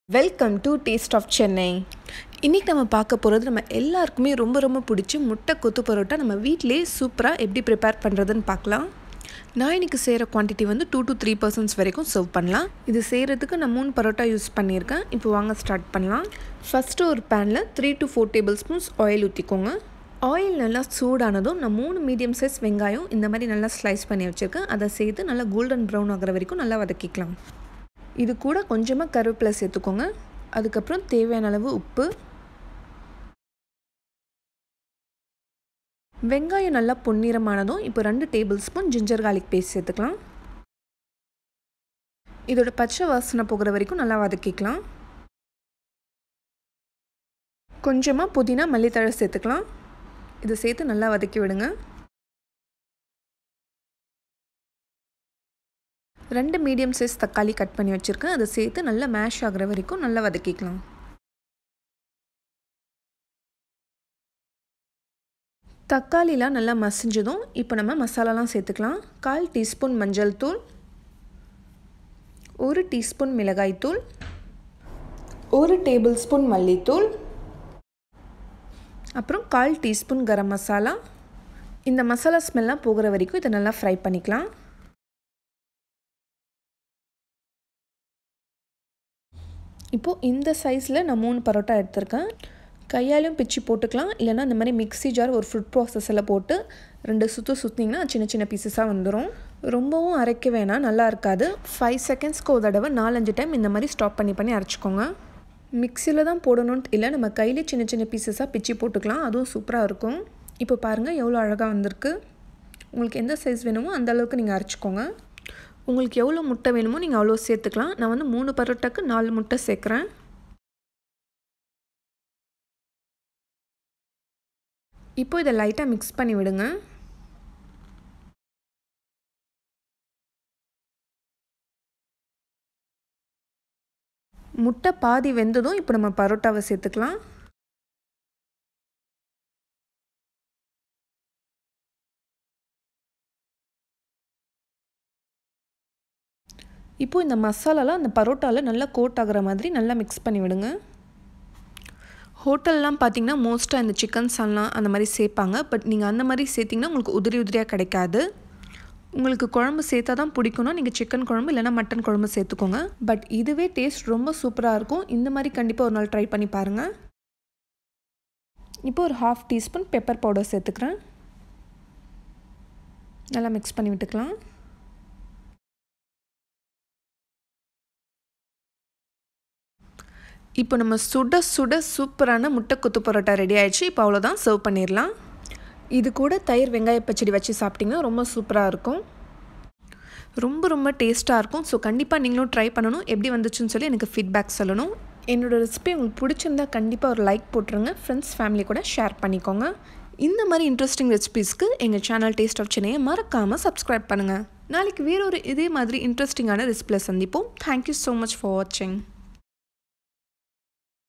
Egg Kothu Parotta in Tamil | Muttai Parotta | Kothu Parotta Recipe | Famous Street Food
Kothu Parotta is the popular street food of Tamil Nadu. It is the best dish to make with leftover parottas.. Try this amazing recipe and give us your feedback...